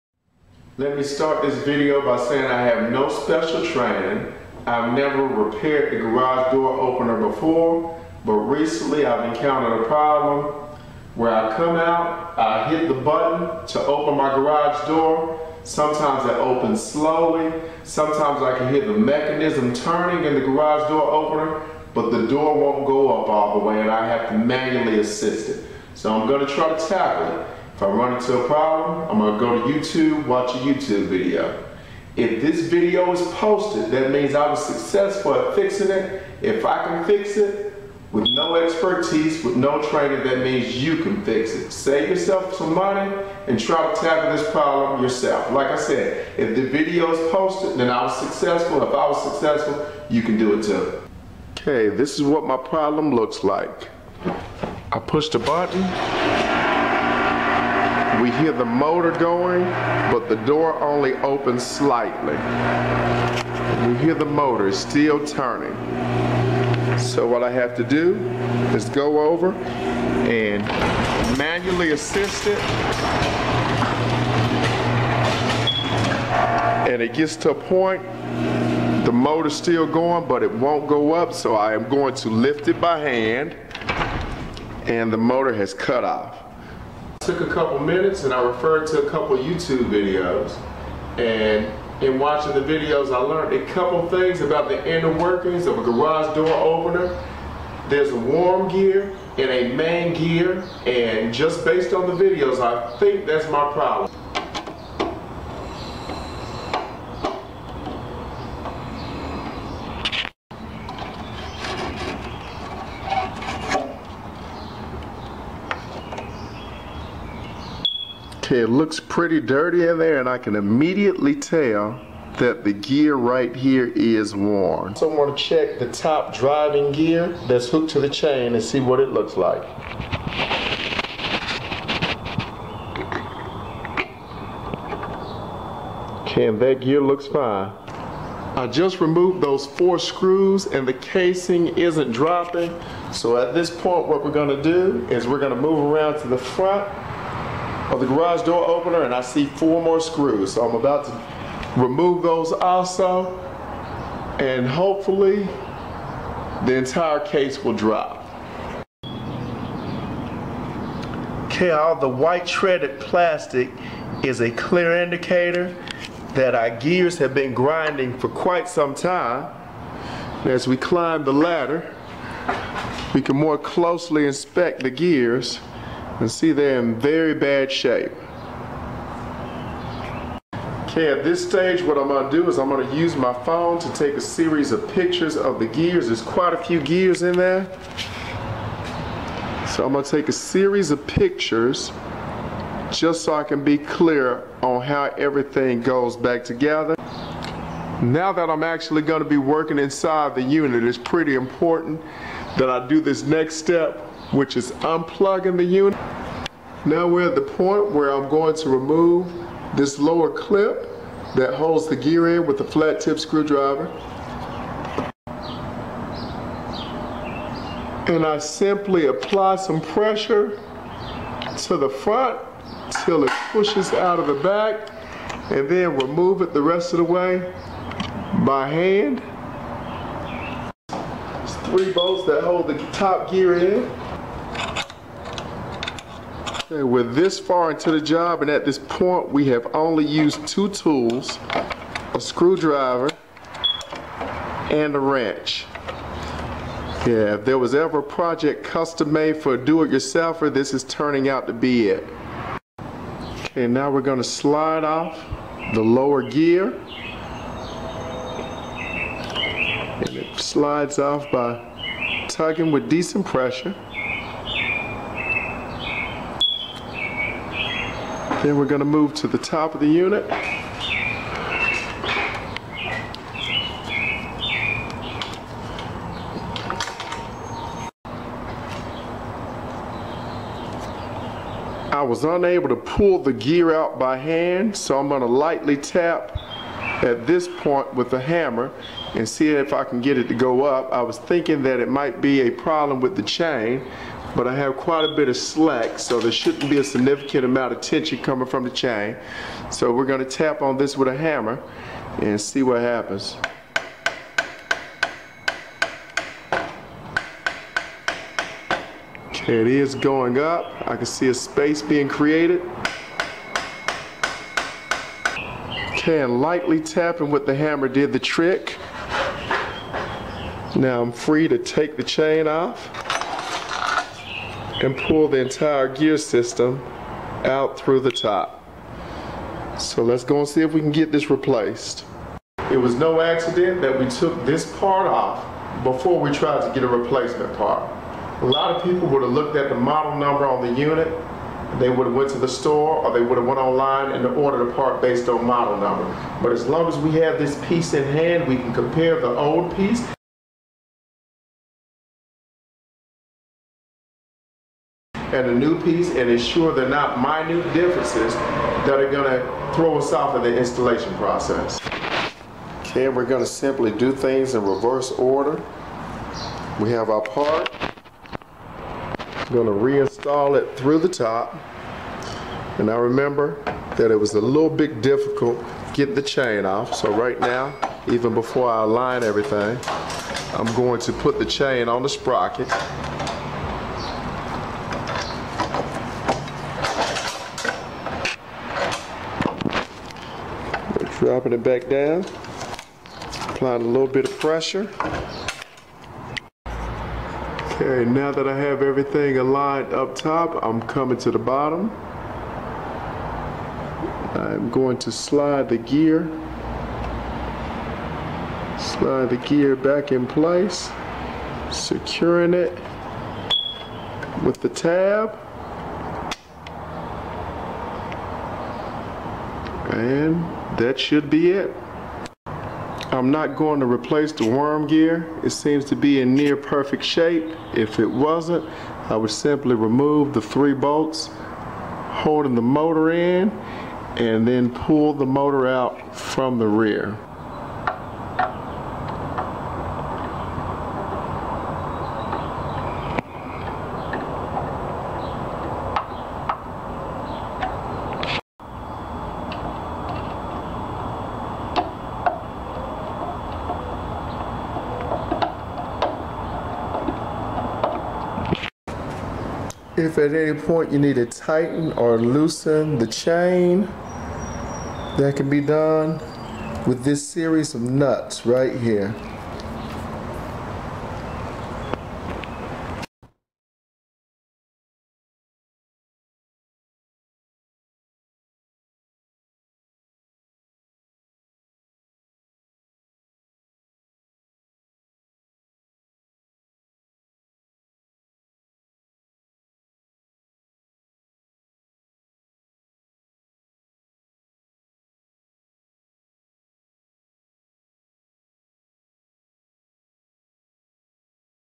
GARAGE DOOR OPENER TROUBLESHOOTING & REPAIR
I show that with no training you diagnose  your garage door opener, and repair it quickly.